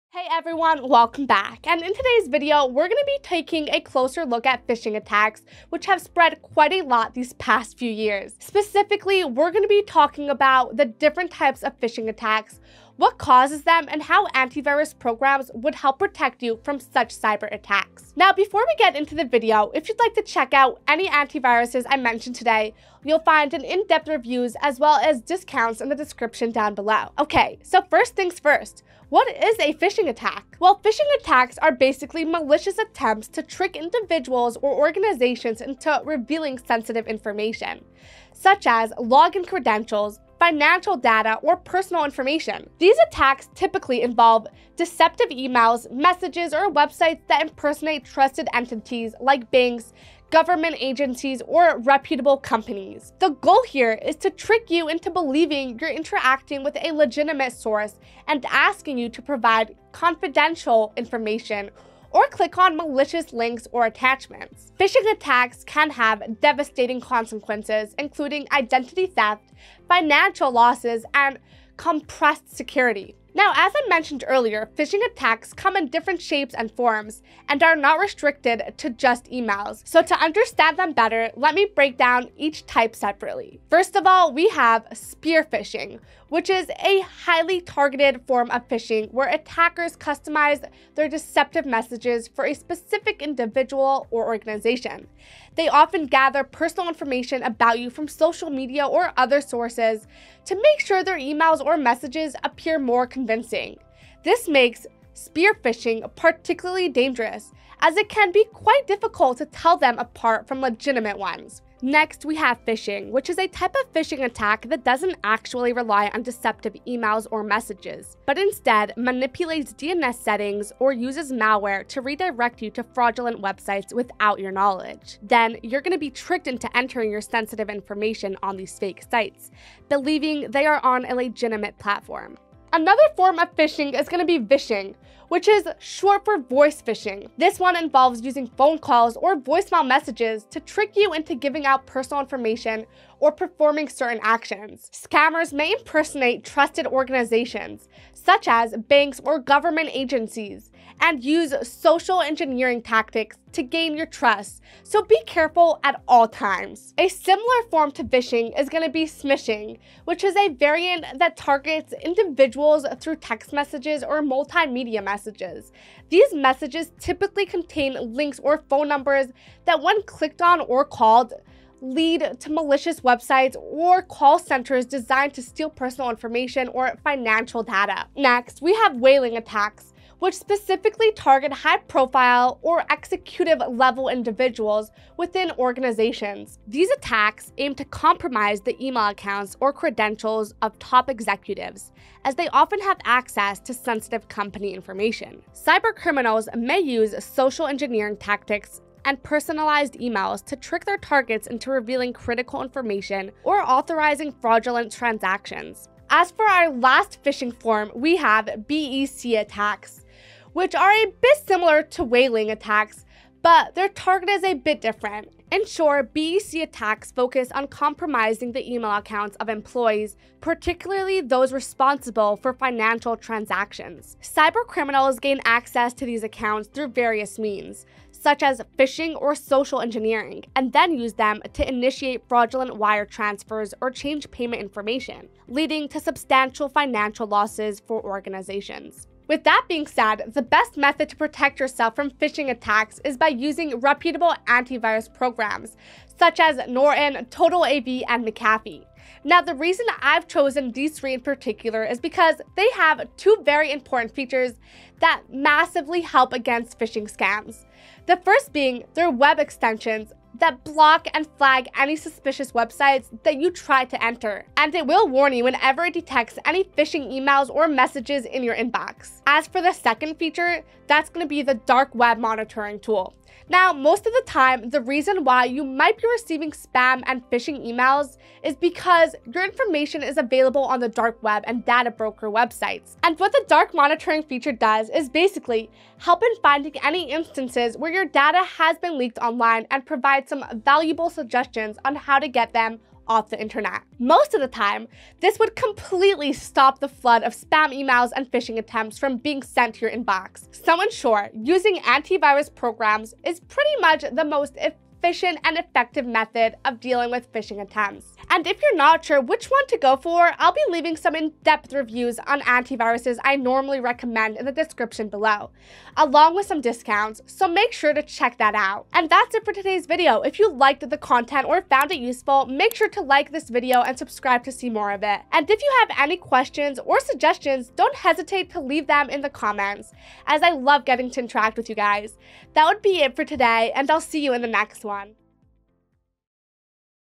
Phishing Attacks And How Antivirus Protects You
Phishing Attacks And How Antivirus Protects You: Discover the ins and outs of phishing attacks and how antivirus software safeguards your online world. Stay informed, stay secure!

Hey everyone, welcome back. And in today’s video, we’re gonna be taking a closer look at phishing attacks, which have spread quite a lot these past few years. Specifically, we’re gonna be talking about the different types of phishing attacks, what causes them, and how antivirus programs would help protect you from such cyberattacks. Now, before we get into the video, if you’d like to check out any antivirus I mention today, you’ll find in-depth reviews as well as discounts in the description down below. Okay, so first things first, what is a phishing attack? Well, Phishing attacks are basically malicious attempts to trick individuals or organizations into revealing sensitive information, such as login credentials, financial data, or personal information. These attacks typically involve deceptive emails, messages, or websites that impersonate trusted entities like banks, government agencies, or reputable companies. The goal here is to trick you into believing you're interacting with a legitimate source and asking you to provide confidential information or click on malicious links or attachments. Phishing attacks can have devastating consequences, including identity theft, financial losses, and compromised security. So to understand them better, lemme break down each type separately. First of all, we have spear phishing, which is a highly targeted form of phishing where attackers customize their deceptive messages for a specific individual or organization. They often gather personal information about you from social media or other sources to make sure their emails or messages appear more convincing. This makes spearphishing particularly dangerous, as it can be quite difficult to tell them apart from legitimate ones. Next, we have phishing, which is a type of phishing attack that doesn't actually rely on deceptive emails or messages but instead manipulates DNS settings or uses malware to redirect you to fraudulent websites without your knowledge.  Then, you’re gonna betricked into entering your sensitive information on these fake sites, believing they are on a legitimate platform. Another form of phishing is gonna be Vishing, which is short for "voice phishing.". This one involves using phone calls or voicemail messages to trick you into giving out personal information or performing certain actions. Scammers may impersonate trusted organizations, such as banks or government agencies, and use social engineering tactics to gain your trust, so be careful at all times. With that being said, the best method to protect yourself from phishing attacks is by using reputable antivirus programs such as Norton, Total AV, and McAfee. Now, the reason I’ve chosen these three in particular is because they have two very important features that massively help against phishing scams. The first being their web extensions that block and flag any suspicious websites that you try to enter. And will warn you whenever it detects any phishing emails or messages in your inbox. As for the second feature, that’s gonna be the “dark web monitoring” tool. Now, most of the time, the reason why you might be receiving spam and phishing emails is because your information is available on the dark web and data broker websites. And what the dark monitoring feature does is basically  help in finding any instances where your data has been leaked online and provide some valuable suggestions on how to get them off the internet. Most of the time, this would completely stop the flood of spam emails and phishing attempts from being sent to your inbox. So in short, using antivirus programs is pretty much the most efficient and effective method of dealing with phishing attempts. And if you’re not sure which one to go for, I’ll be leaving some in-depth reviews on AVs I normally recommend in the description below, along with some discounts, so make sure to check that out. And that’s it for today’s video. And if you have any questions or suggestions, don’t hesitate to leave them in the comments, as I love interacting with you guys.

Hope you enjoyed my Phishing Attacks And How Antivirus Protects You Video.